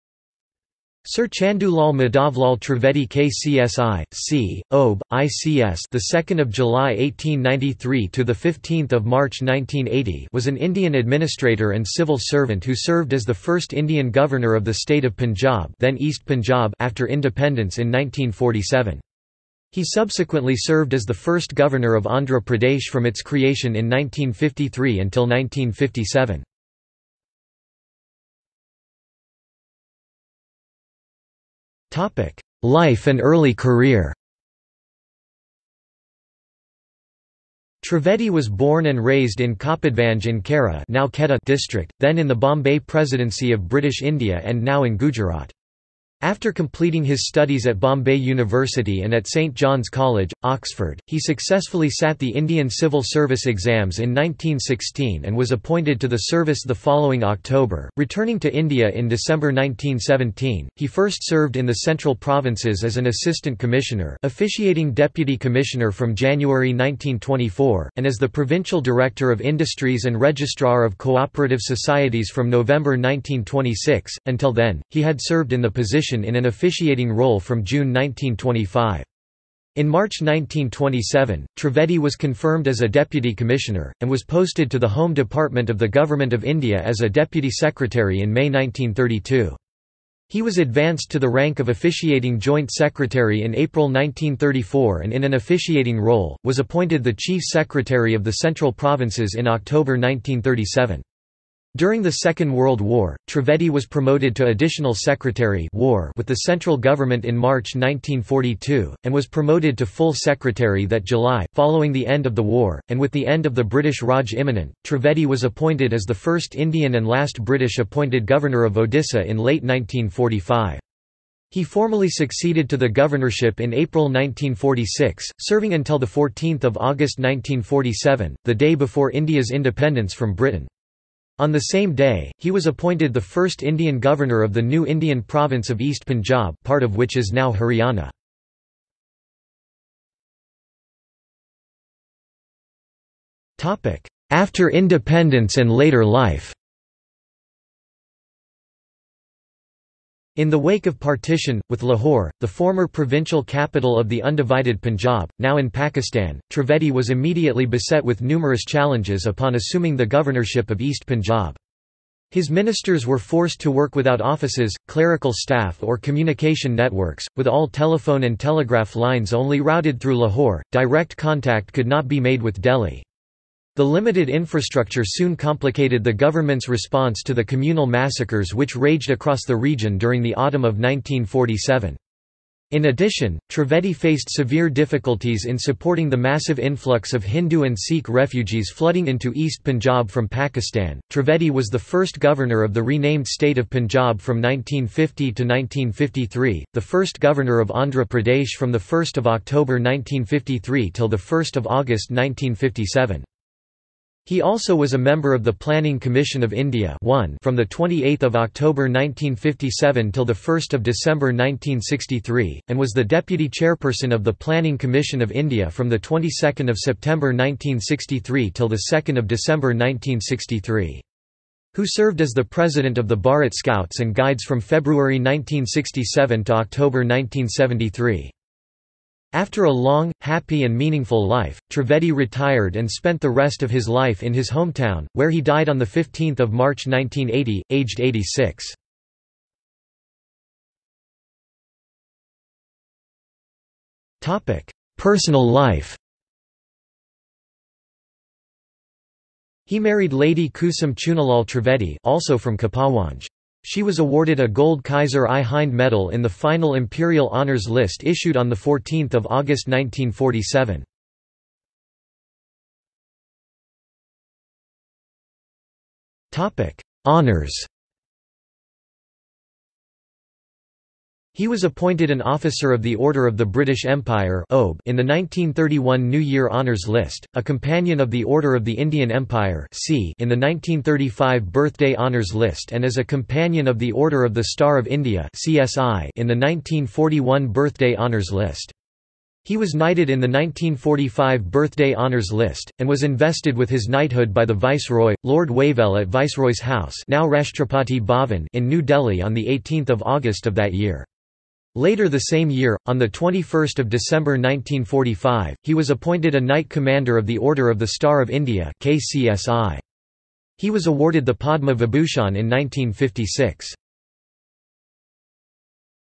Chandulal Madhavlal Trivedi | Wikipedia audio article
This is an audio version of the Wikipedia Article:
Chandulal Madhavlal Trivedi

00:00:28 1 Life and early career
00:02:20 2 After Independence and later life
00:04:00 3 Personal life
00:04:22 4 Honours

- Socrates

SUMMARY
Sir Chandulal Madhavlal Trivedi KCSI, CIE, OBE, ICS (2 July 1893 – 15 March 1980) was an Indian administrator and civil servant who served as the first Indian governor of the state of Punjab (then East Punjab) after Independence in 1947. He subsequently served as the first Governor of Andhra Pradesh from its creation in 1953 until 1957.